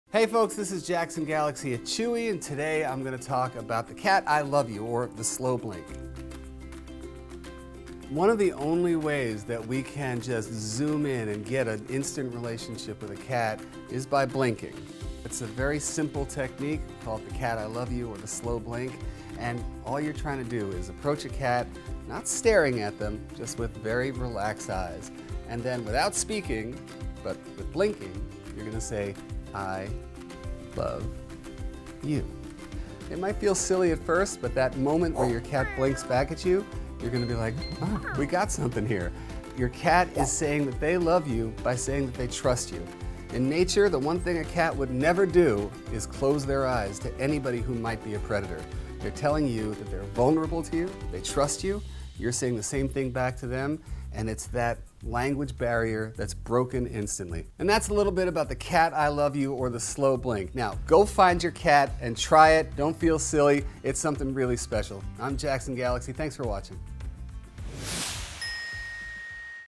Jackson Galaxy Reveals How To Tell Your Cat "I Love You" | Chewy
Want to know how to make your cat love you even more? Well first you’ve gotta speak cat language. And cat love language is a little more different than you think. That’s because sometimes it doesn’t any distinct words or cat behavior. Only actions. Understanding cats is to know that that cats are always on the lookout, so they just don’t close those cat eyes just for anybody or anyone. You can even buy them all the fancy cat supplies and treats you can find at Chewy.com to make their hearts explode with joy. But what if they knew how much you loved them just as much as they love you? We know know you can verbally say “I love my cat so much” but is that really how to speak to cats? Luckily, Jackson Galaxy has cat psychology down-pat. Jackson Galaxy introduces the slow-blink method. Who knew something so subtle could mean so much to our feline friends. Now there’s never an awkward pause after you tell your kitties “I love my cat so much”. If your kitty mimics this special slow-blink, those are signs your cat loves you back. This is a huge way of her telling you the feelings of trust and love are mutual. And remember, every feline has different cat personality and cat characteristics. Sometimes all it takes is a little patience and persistence for your kitty to finally know how to speak to cats. Although there may be more than 100 ways to love a cat, Jackson Galaxy explains how to tell your cat “I love you” in this video!

Shop all your favorite cat supplies and more at Chewy.com today!